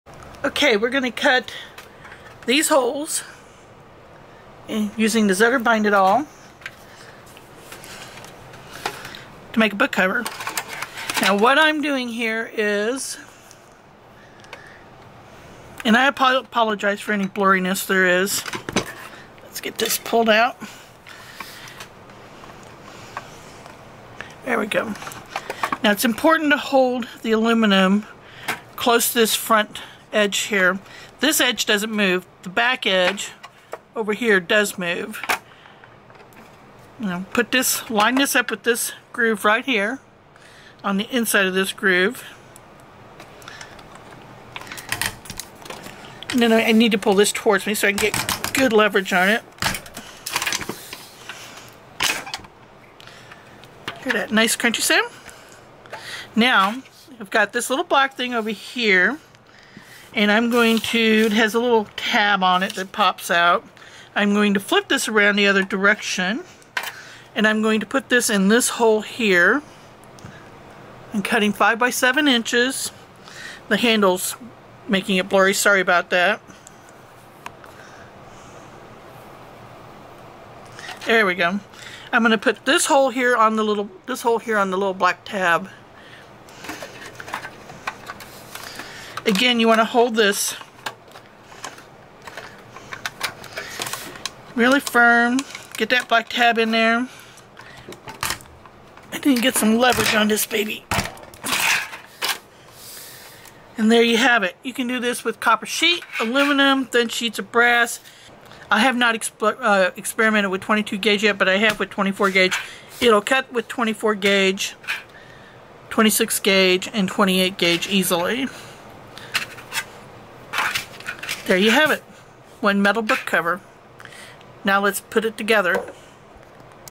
Using Aluminum Flashing for Book Covers!
This video focuses on how I cut the holes in the aluminum flashing so I can use it as a book/journal cover!

Items mentioned in the video:
Zutter bind-it-all
Zutter O-wire (this comes in several sizes and colors)
Sizzix Big Shot.
Cutting Dies Stencil Metal Template Molds.
Irwin Marples 1/4" Wood chisel
Irwin Marples 1/8" wood chisel
5 x 7 inch canvas panels
Canson watercolor paper 9 x 12 inches
9 x 12 inch chip board for book binding
White painters tape
We R Memory Keepers Corner Chomper
Heavy Duty Shears for thin gauge metal
Aluminum Flashing 14" wide x 10 feet

I buy my copper sheet from Monsterslayer.com